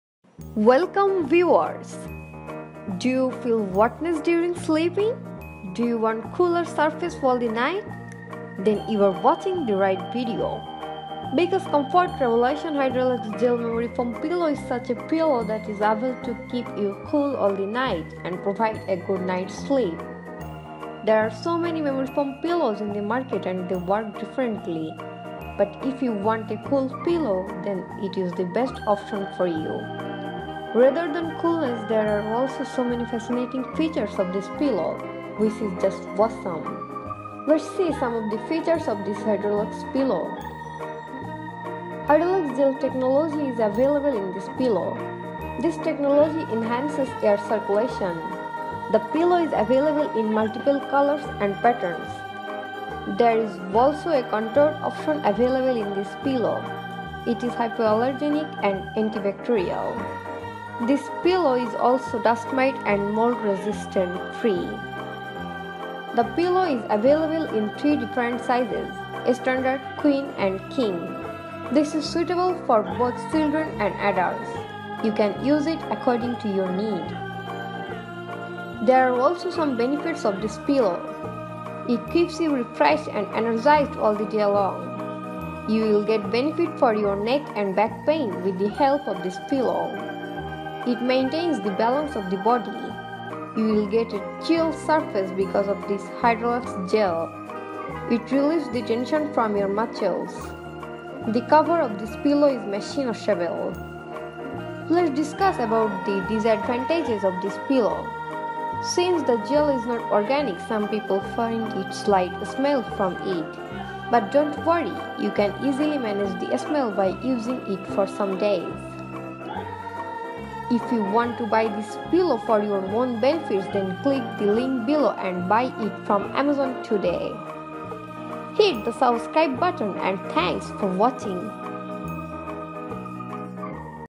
Comfort Revolution Hydraluxe Gel Memory Foam Pillow Review and buying Guide
Do you feel hotness during sleeping? Do you want cooler surface all the night? Then you are watching the right video. Because Comfort Revolution Hydraluxe Gel Memory Foam pillow is such a pillow that is able to keep you all the night and provide a good night’s sleep. There are so many memory foam pillows in the market and they work differently. But if you want a cool pillow then it is the best option for you. Rather than coolness, there are also so many fascinating features of this pillow which is just awesome. Let’s see, some of the features of this hydraluxe pillow.

Hydraluxe gel technology is available in this pillow.
This technology enhances air circulation.
The pillow is available in multiple colors and patterns.
There is also a contoured option available in this pillow.
It is hypoallergenic and anti-bacterial.
This pillow is also dust mite and mold resistant.

The pillow is available in three different sizes- standard, queen, and king. This is suitable for both children and adults. You can use it according to your need.

Benefits
It keeps you refreshed and energized all the day long.
You’ll get the benefit for your neck and back pain with the help of this pillow.
It maintains the balance of the body. You will get a chilled surface because of this hydraluxe gel.
It relieves the tension from your muscles.
The cover of this pillow is machine washable.

Let’s discuss the disadvantages of this pillow.
Since the gel is not organic some people find a slight smell from it. But don’t worry, you can easily manage the smell by using it for some days.

If you wanna learn more about this Comfort Revolution Hydraluxe Gel Pillow,